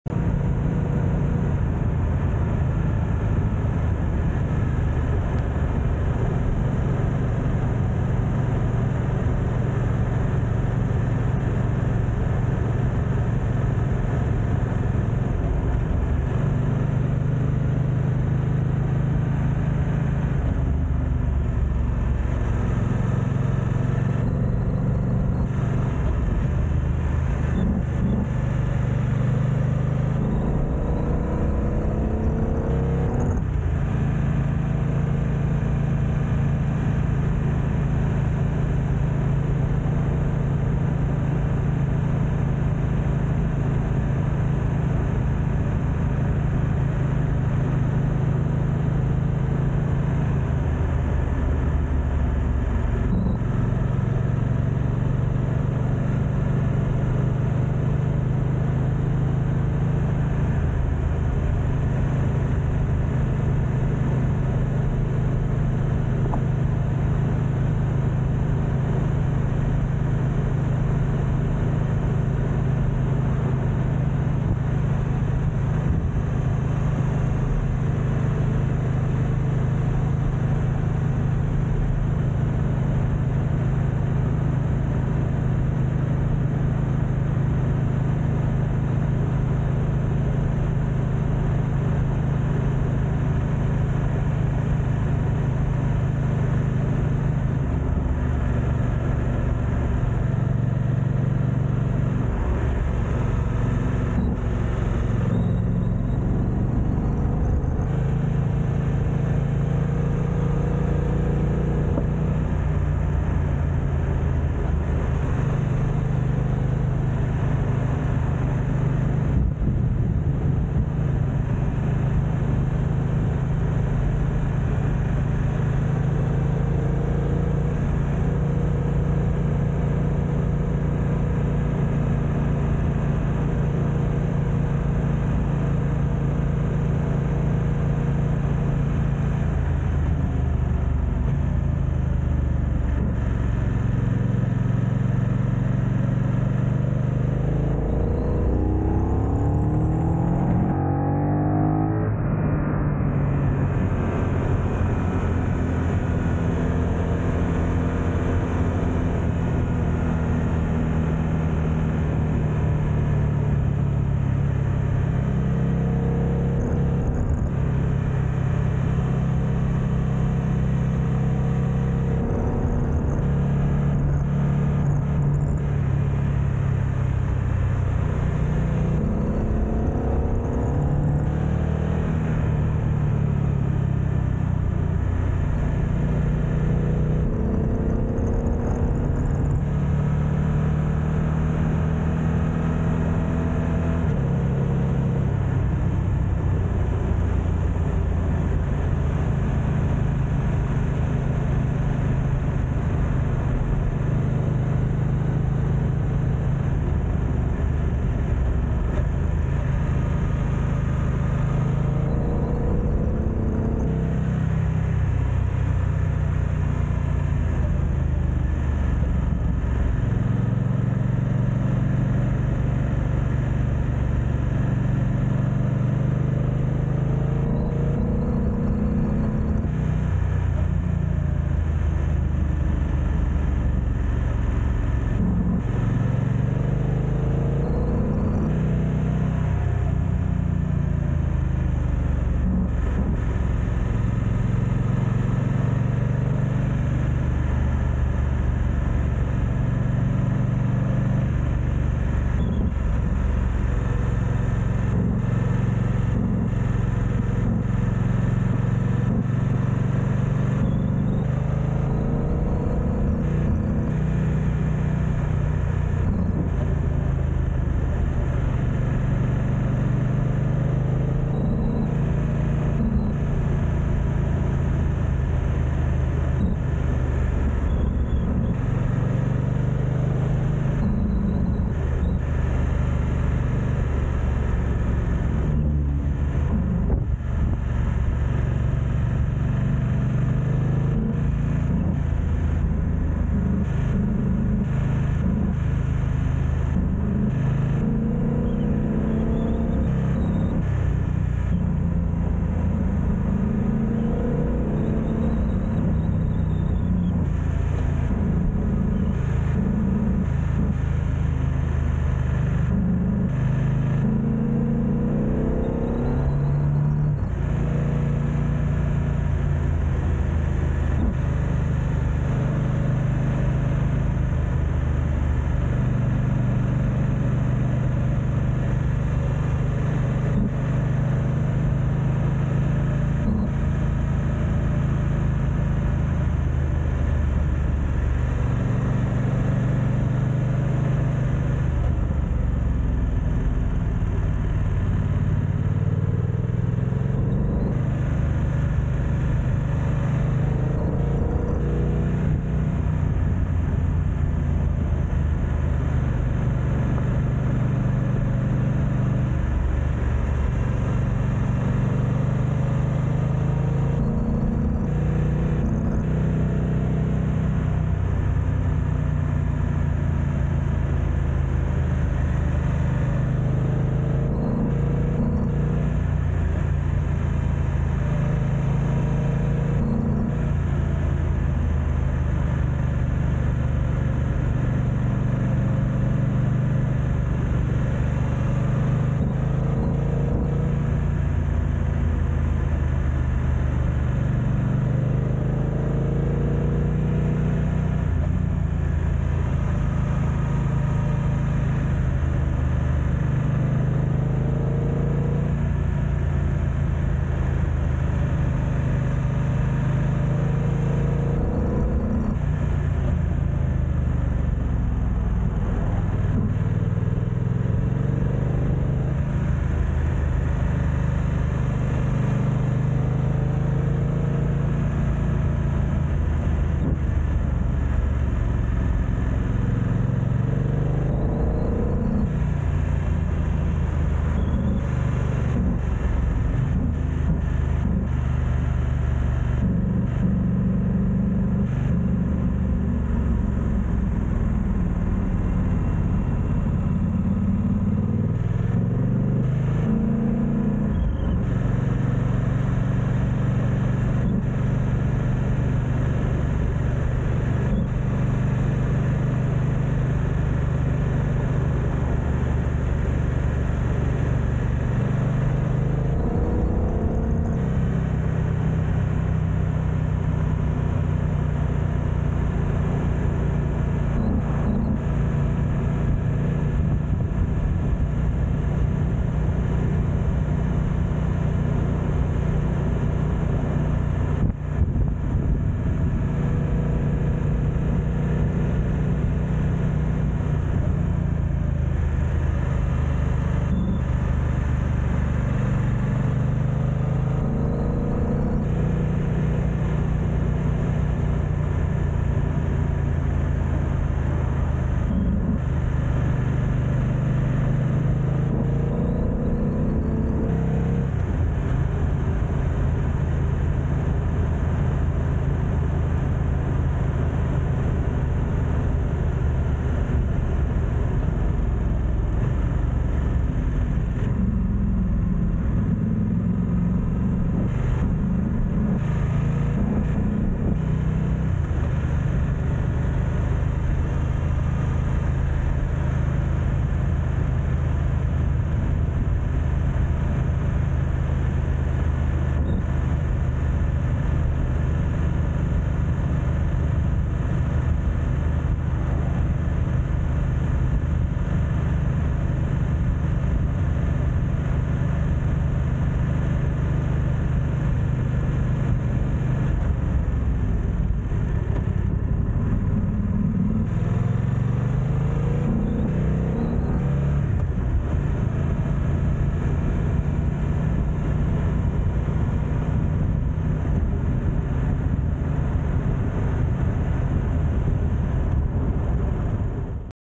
The F900XR sounds meaty when going slow
Raw footage from a recent ride up Mt Tamborine. Wind noise is a real problem with the one x2's internal microphones, but it does an ok job.